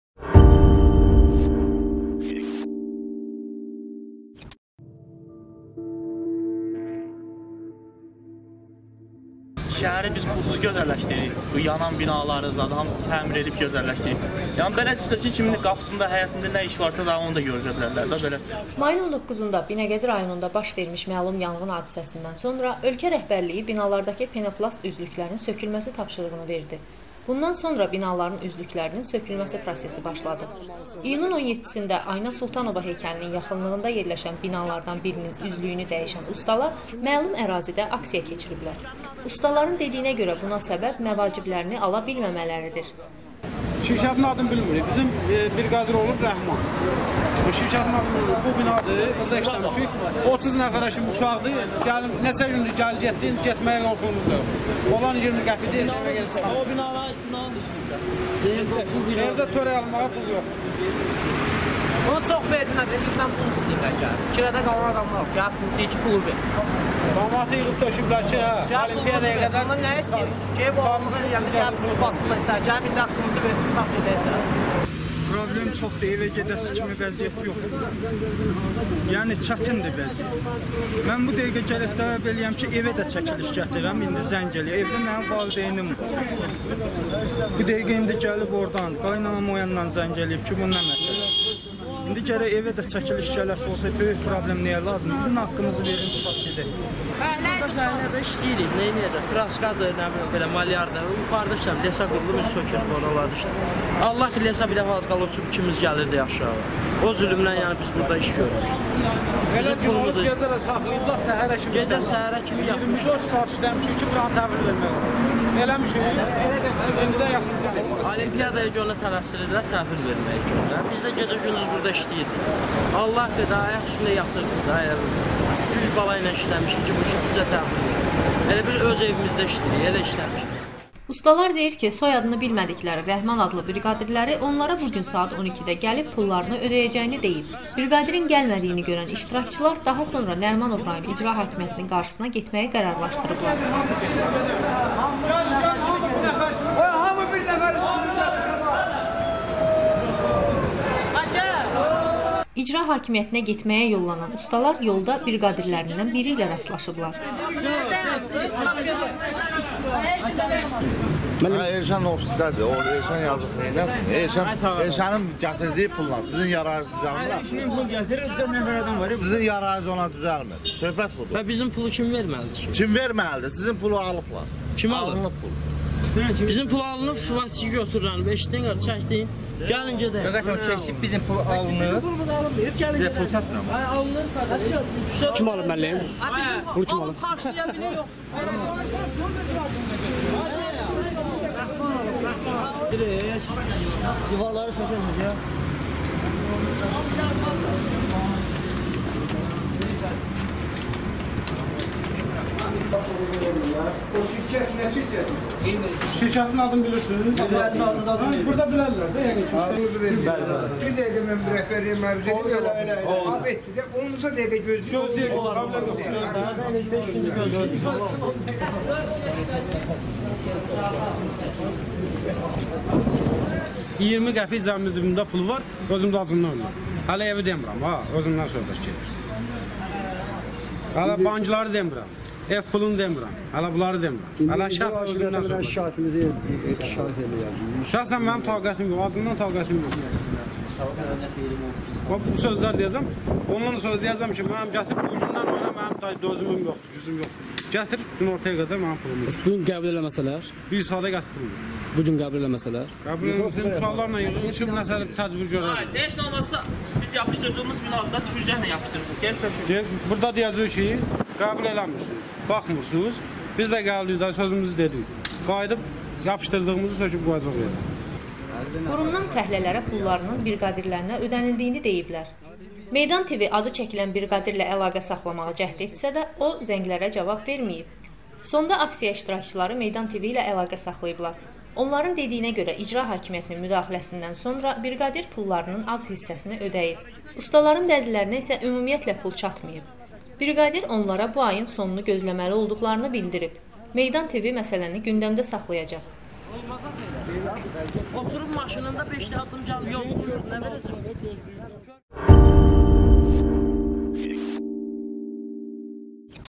Üzlük dəyişən ustalar aksiya keçirdilər
Ətraflı OXU:
Mayın 19-da Binəqədi rayonunda baş vermiş məlum yanğın hadisəsindən sonra ölkə rəhbərliyi binalardakı penaplast üzlüklərin sökülməsi tapşırığını verdi. Bundan sonra binaların üzlüklərinin sökülməsi prosesi b aşladı. İyunun 17-də Ayna Sultanova heykəlinin yaxınlığında yerləşən binalardan birinin üzlüyünü dəyişən ustalar məlum ərazidə aksiya keçiriblər. Ustaların dediyinə görə, buna səbəb  məvaciblərini ala bilməmələridir.

Ustalar deyir ki, soyadını bilmədikləri Rəhman adlı briqadirləri onlara bu gün saat 12-də gəlib, pullarını ödəyəcəyini deyib. Briqadirin gəlmədiyini görən iştirakçılar daha sonra Nərimanov Rayon İcra Hakimiyyətinin qarşısına getməyi qərarlaşdırıblar. İcra hakimiyyətinə yollanan ustalar yolda briqadirlərindən biri ilə rastlaşıblar. Briqadir onlara pullarının alındığını deyib. Lakin pulları kimin almasına açıqlama gətirməyib.
Nərimanov Rayon İcra Hakimiyyətindən ustalara  pullarının briqadirlərinə ödənildiyini deyiblər.
Meydan Tv adıçəkilən briqadirlə əlaqə saxlamağa cəhd etsə də, o, zənglərə cavab verməyib.
 Sonda aksiya iştirakçıları Meydan Tv-ilə əlaqə saxlayıblar. Onların dediyinə görə, icra hakimiyyətinin müdaxiləsindən sonra briqadir pullarının az hissəsini ödəyib. Ustaların bəzilərinə isə, ümumiyyətlə, pul çatmayıb. Briqadir onlara bu ayın sonunu gözləməli olduqlarını bildirib. Meydan TV məsələni gündəmdə saxlayacaq

Audio: